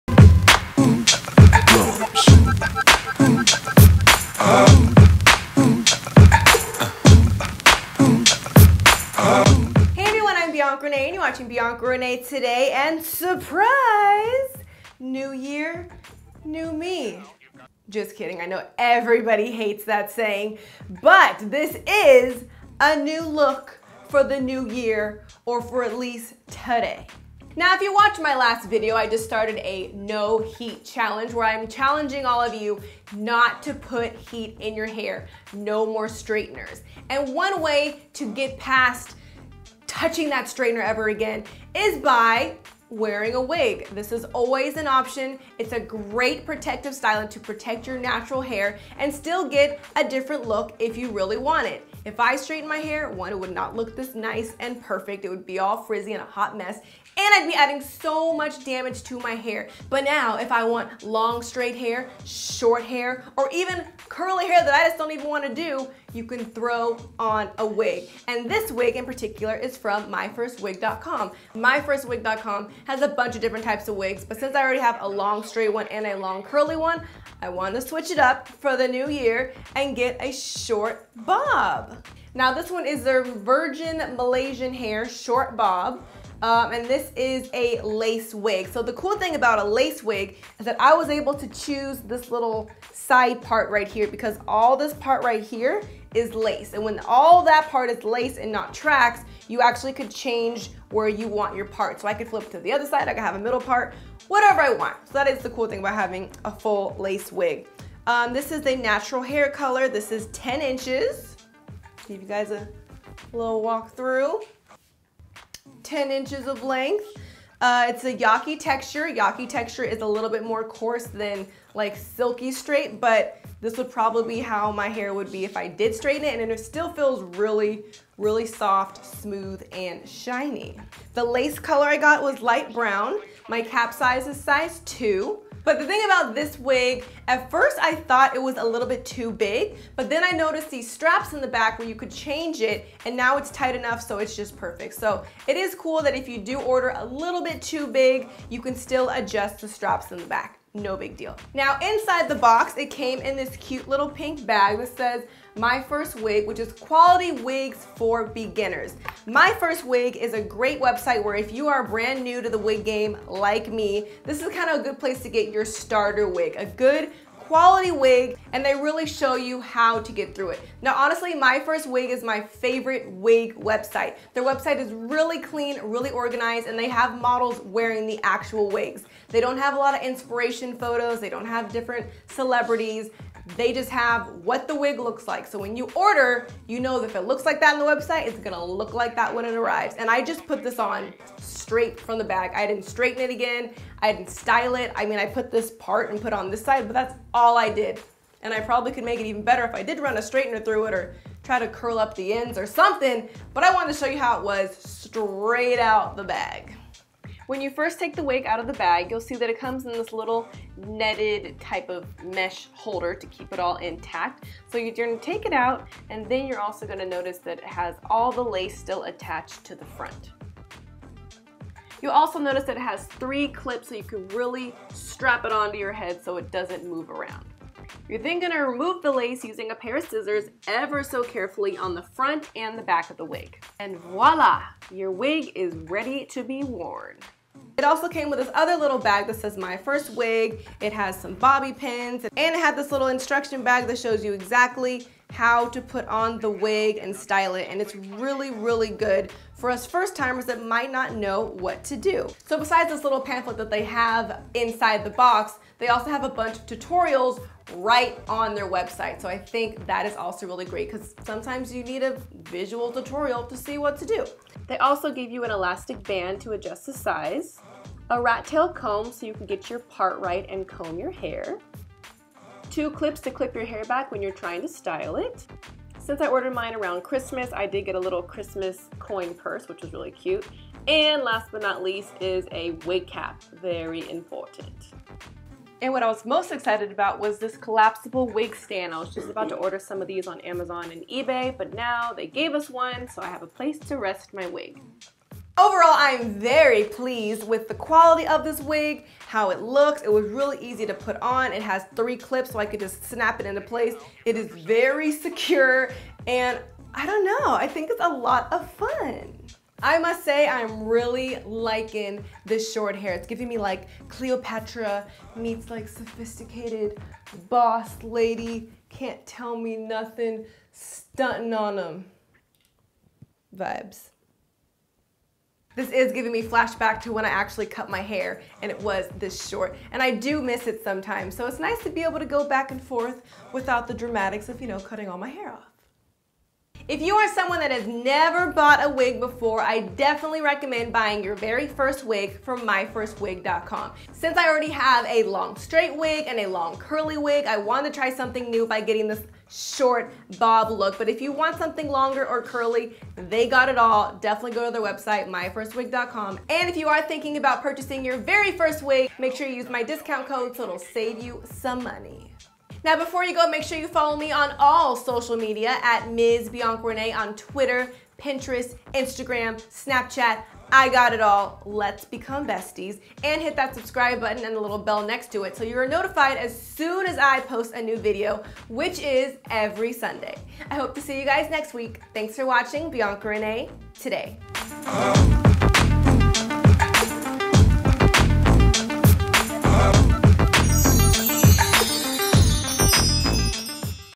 Wigs For Beginners! MyFirstWig.com| BiancaReneeToday
MyFirstWig.com is a GREAT website for wig beginners! Their website is very clean and organized and has many example videos explaining how to put on and care for your wigs. MyFirstWig is also the sister company to RPG Show wigs so you know you are getting TOP NOTCH wigs!

+ a FREE WIG KIT!

Hair product : Victoria - Virgin Malaysian Hair Short Bob Lace Wig - MFB003
  - Hair Color: Natural Hair Color
  - Length: 10 Inch
  - Texture: Yaki
  - Lace Color: Light Brown
  - Cap Size: Size 2
  - Wig Cap Color: Brown
  - Cap Construction: Glueless Lace Cap

$10 OFF + FREE wig kit!!